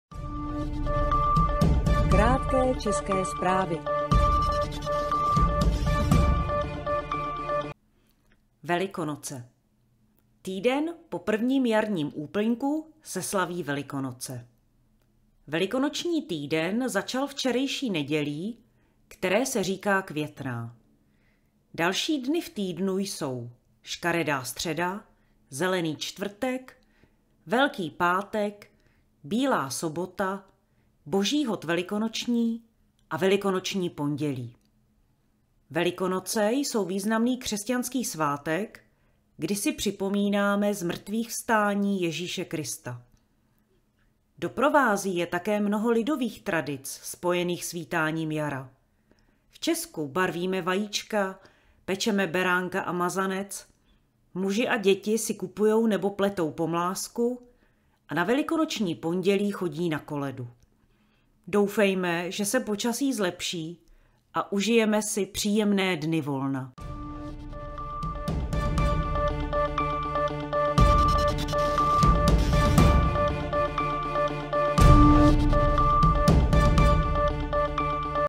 Velikonoce
Poslouchejte krátké české zprávy, dnes na téma: Velikonoce.

Titulky můžete zapnout nebo vypnout – klikněte na ikonu titulků (nebo použijte klávesu C).
Zdroj fotografií:
Pixabay: Thomas
WikimediaCommons: Josef Plch

Příjemný poslech!